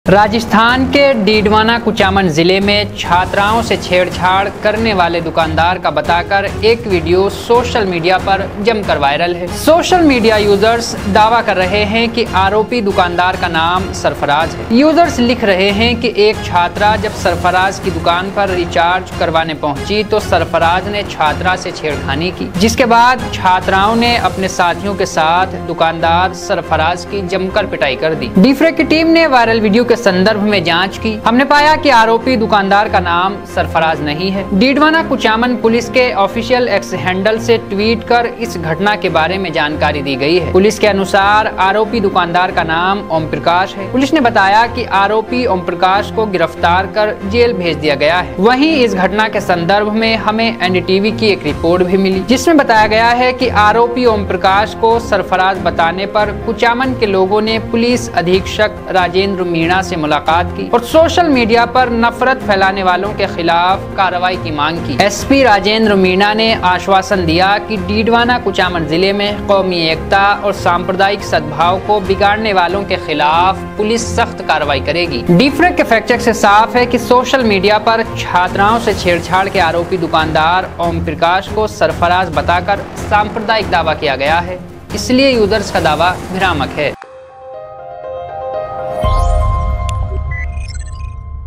Accused of molesting girl studentsViral as Muslim||Fact check || DFRAC#Rajasthan
Team Dfrac

Report By:-
Anchor:-

ALL THE SOCIAL MEDIA PLATFORM